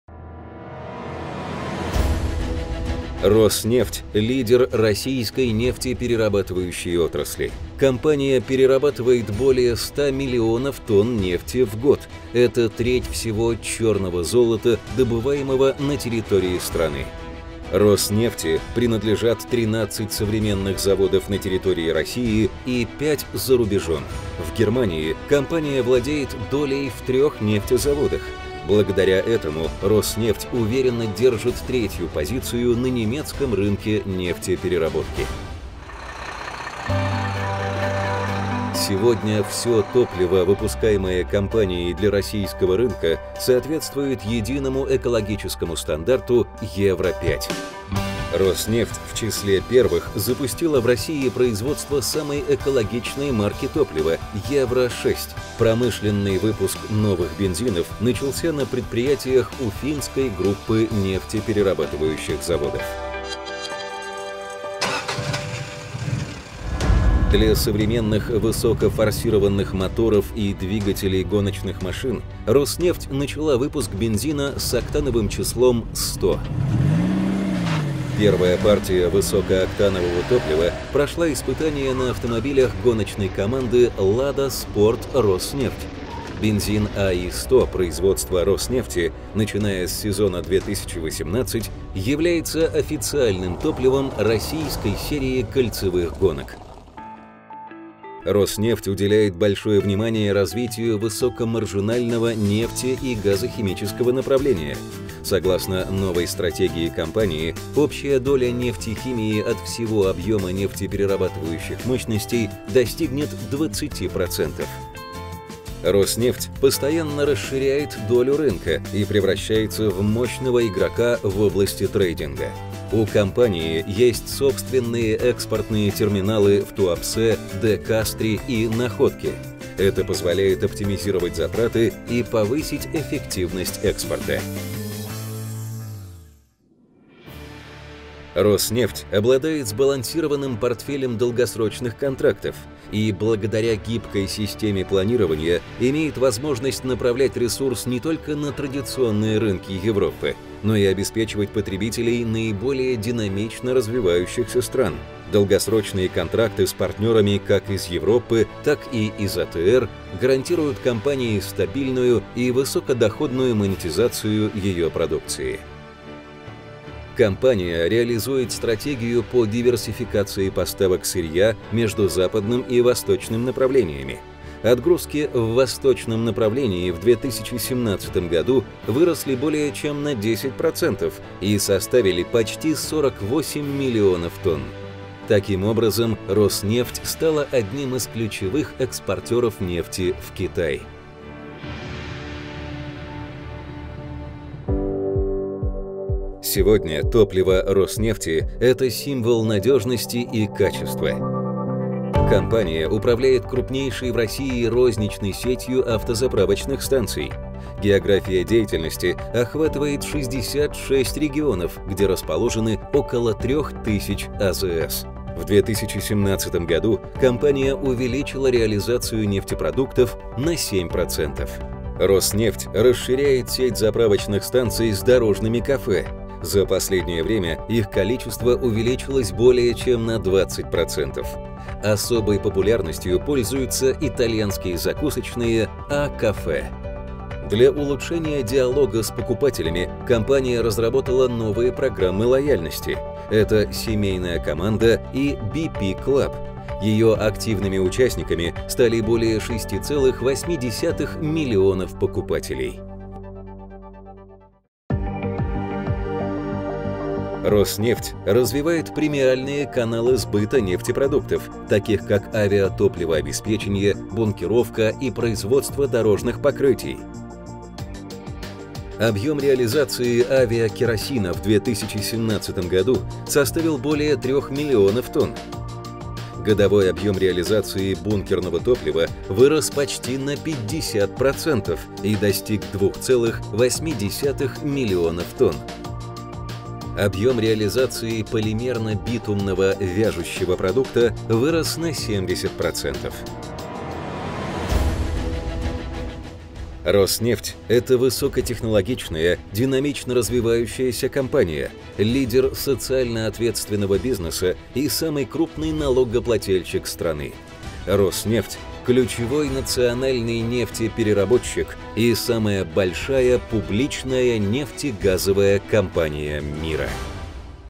Роснефть на рынке нефтепераработки
Компания перерабатывает более трети добываемой на территории России нефти - более 100 млн тонн в год.

Роснефти принадлежат 13 современных заводов на территории России и 5 - за рубежом (в том числе в Германии и Индии).